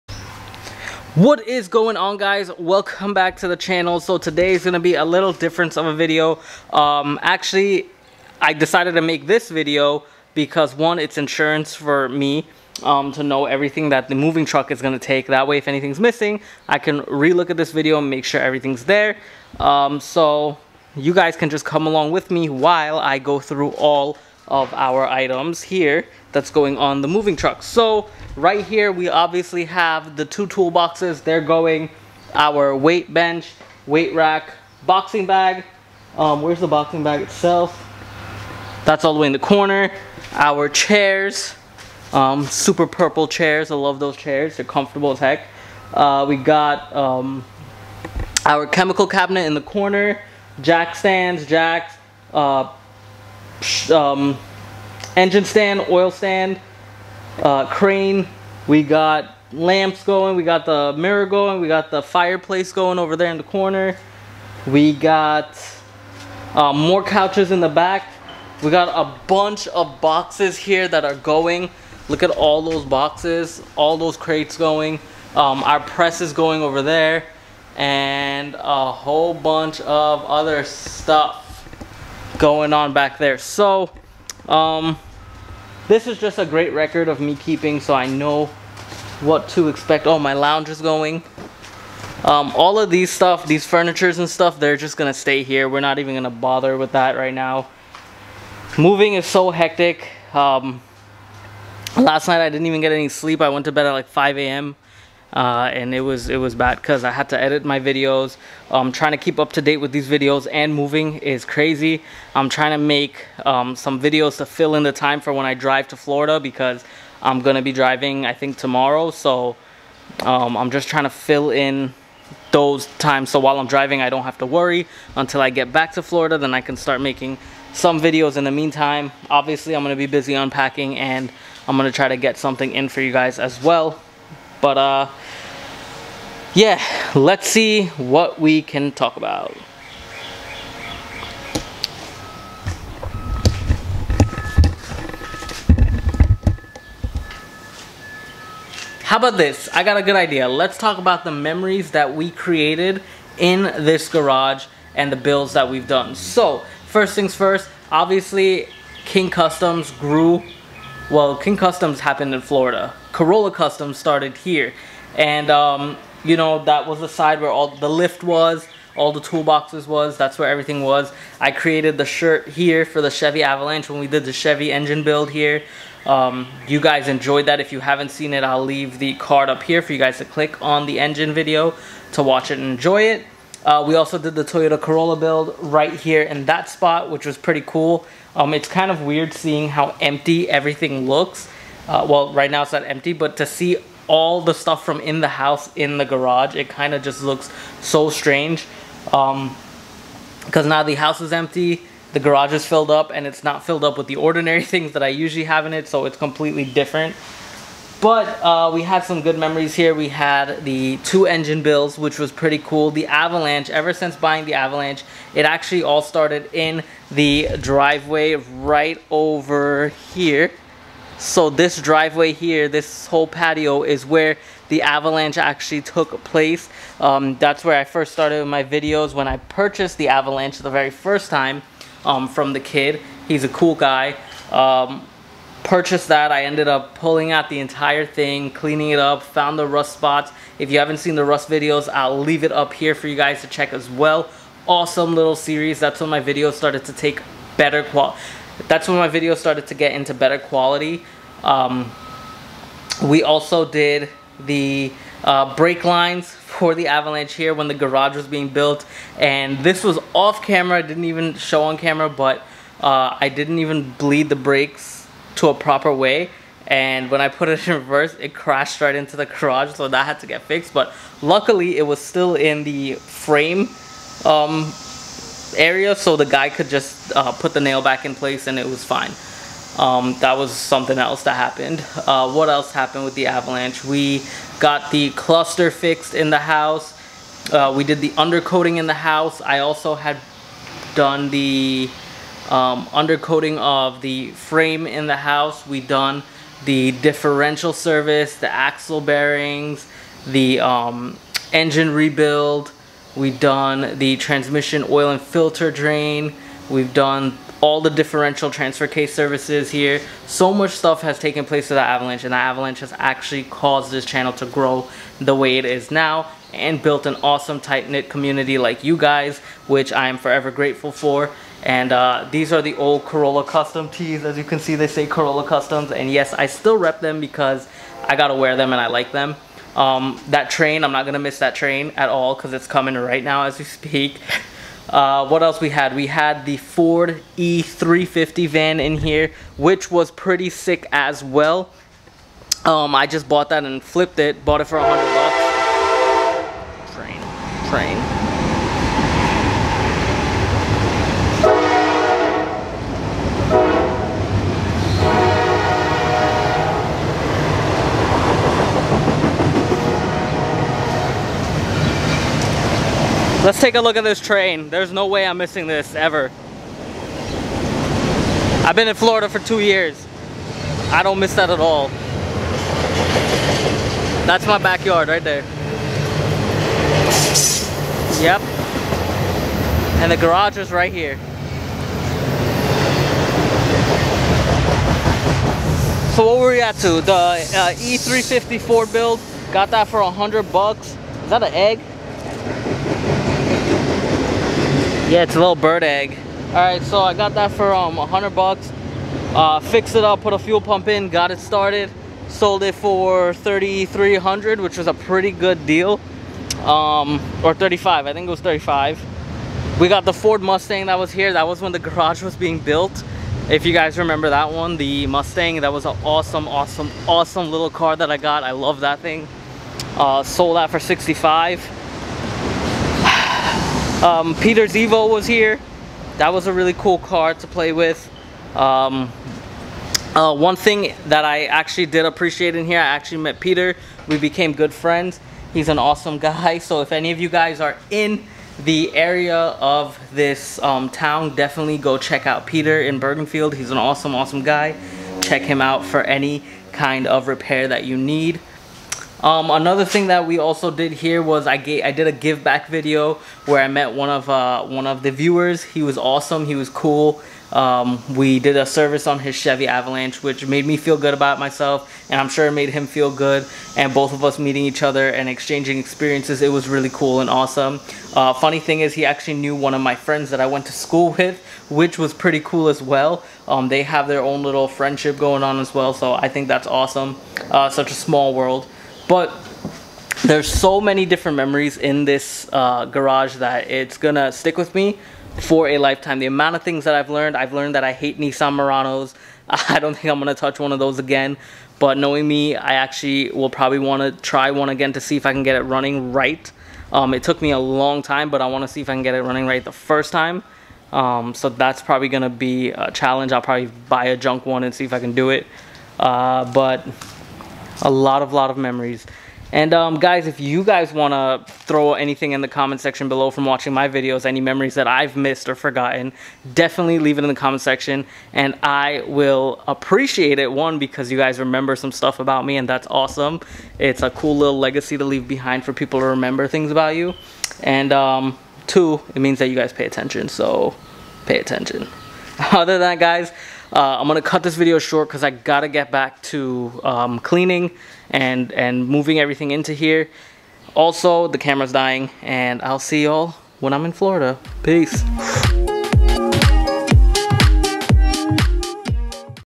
moving day is here, remembering the beginning
in this video, we talk about the memories of corolla customz into king customz. walk down memory lane with me please. lets get it.